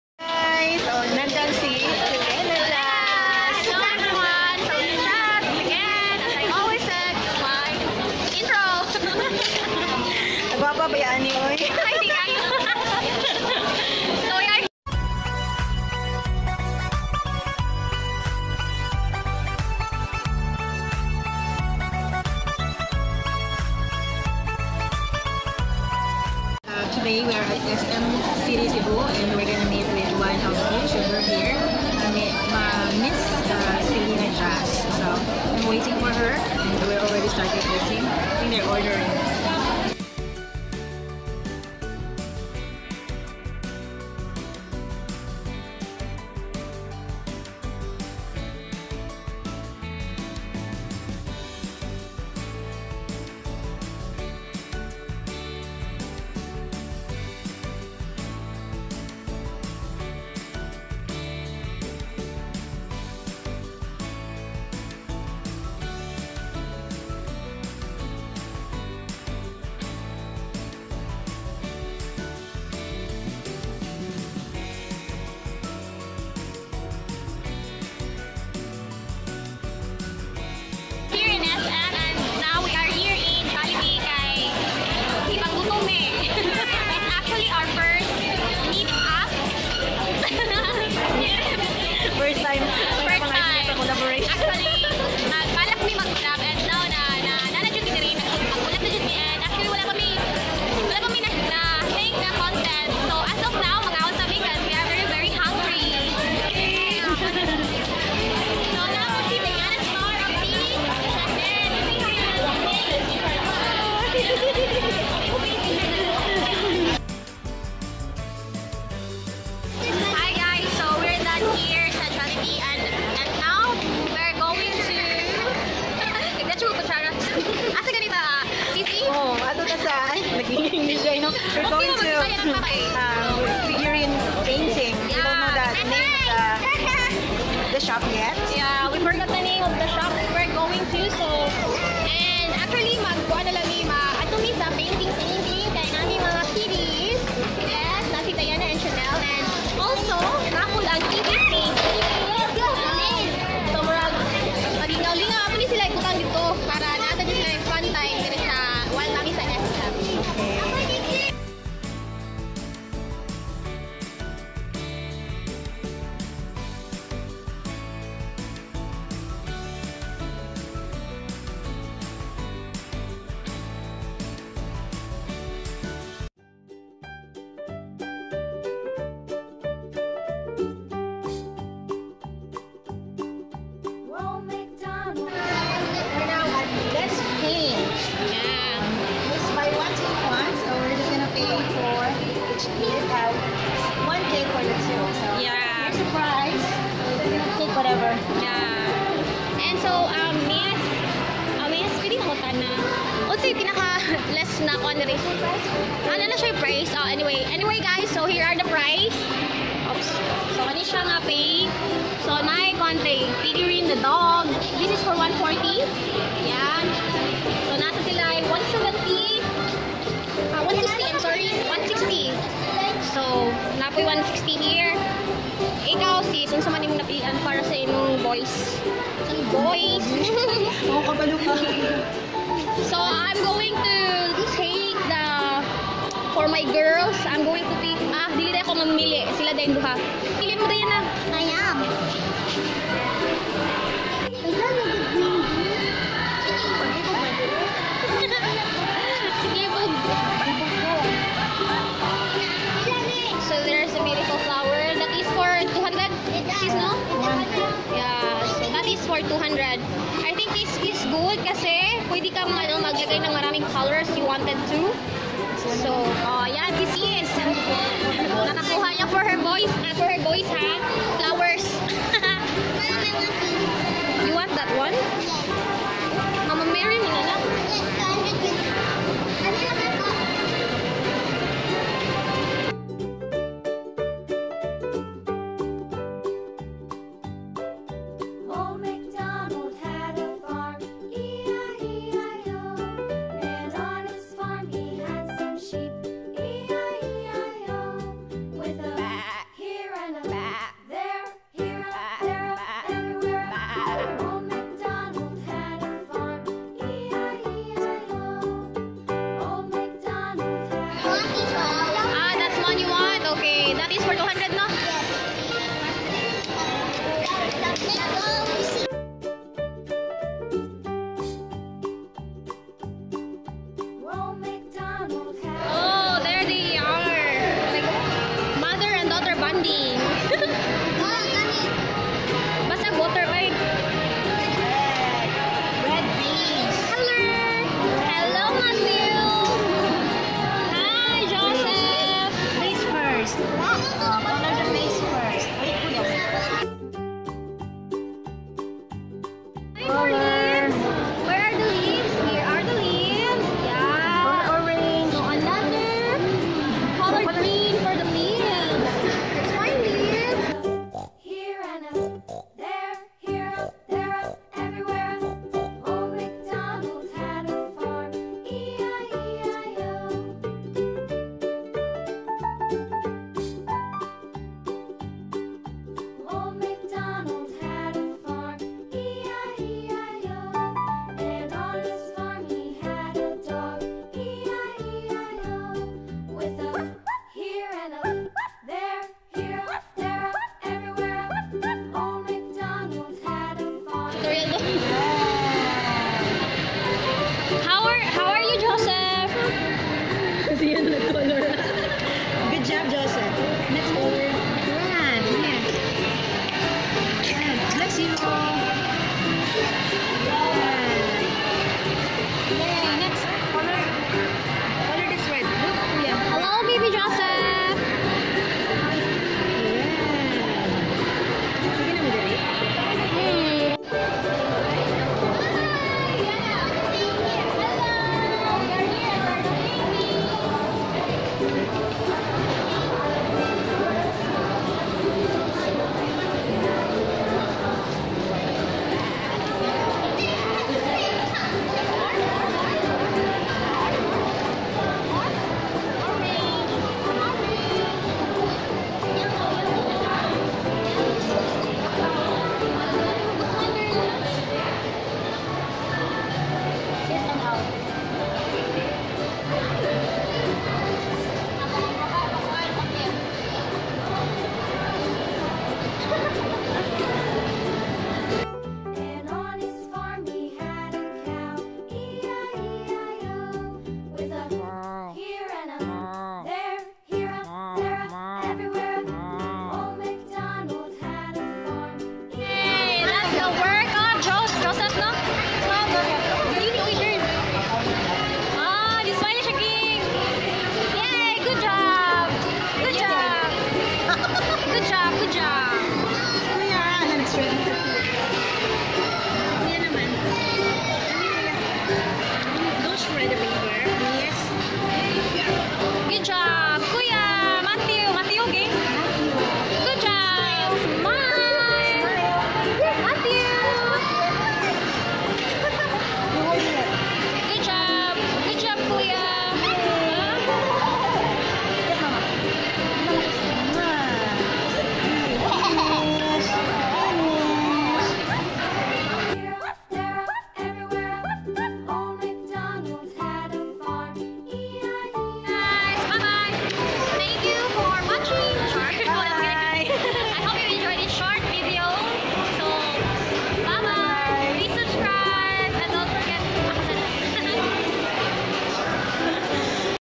Celena Jas Collaboration| First Meet up| Let's Paint SM Cebu
First meet up and collaboration of Youtuber
Having lunch at Jollibee SM Cebu and kids painting at LetsPaint